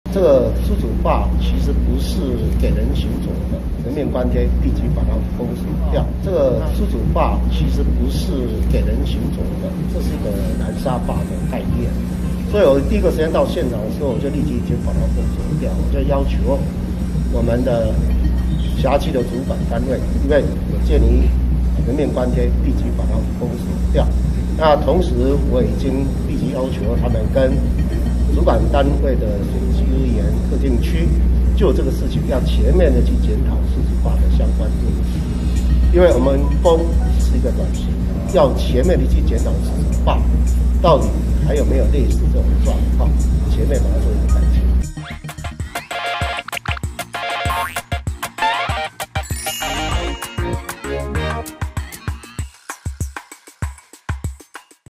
虎豹潭奪4死2失蹤！侯友宜下令「封鎖梳子壩」：全面檢討、改進
記者 陳以昇 拍攝